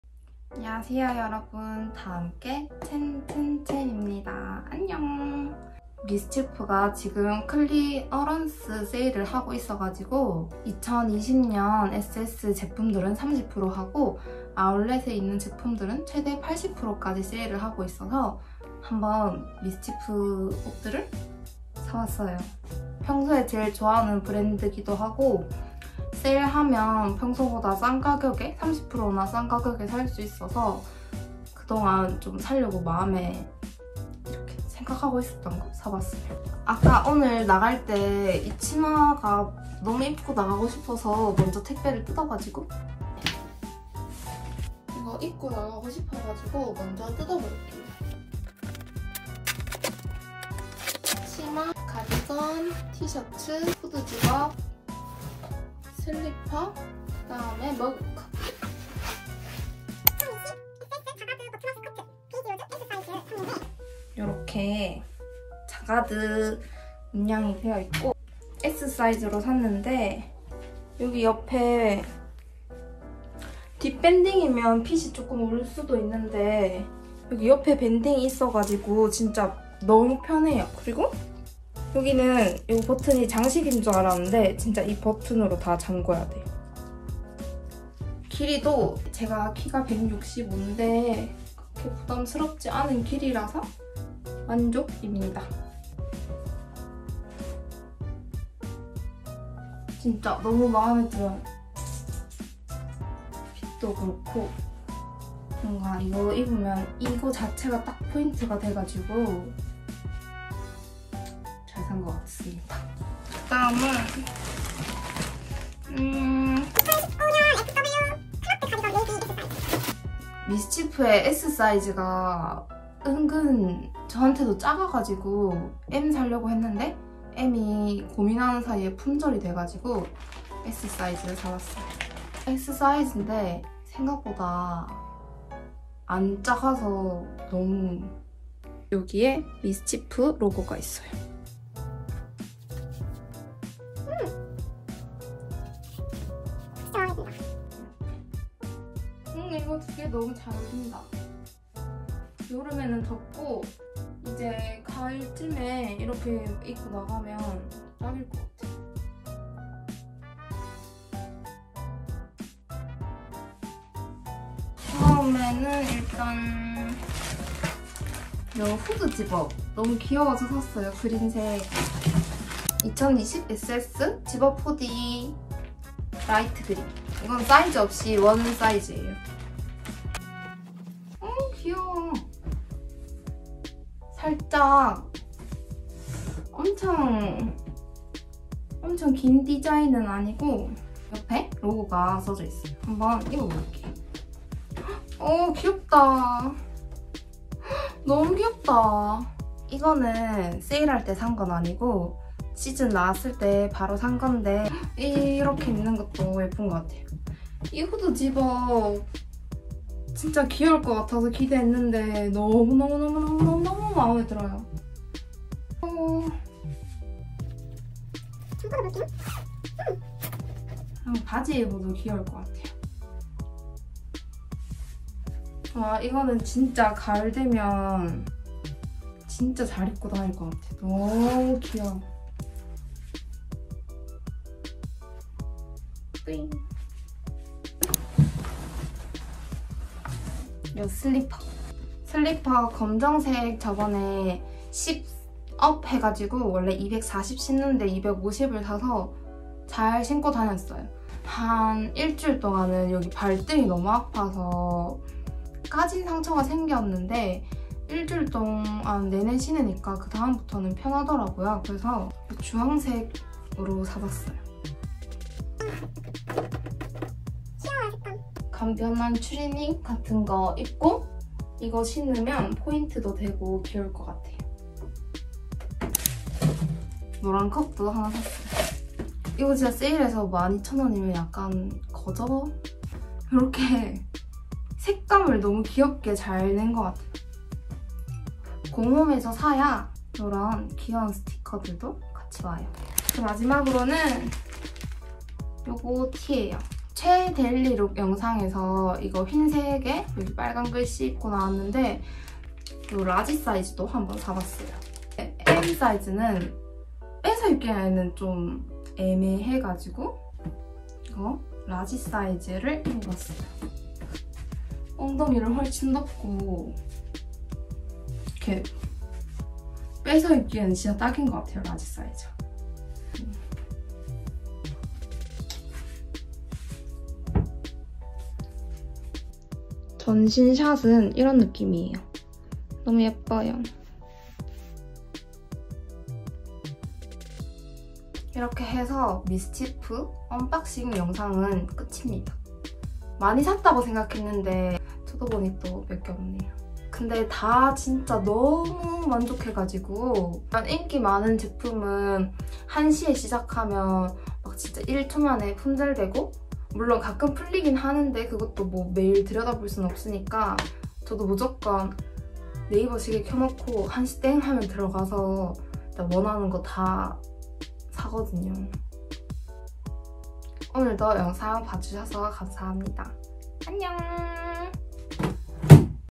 미스치프 클리어런스 세일 언박싱 | MISCHIEF CLEARANCE SALE UNBOXING
안녕하세요 여러분 다함께 챈챈챈입니다~
제 최애 브랜드인 미스치프에서 클리어런스 세일을 해서 언박싱 영상 들고왔어요••
품절된 제품도 많지만 아직 남아 있는 제품 들도 있어요🤍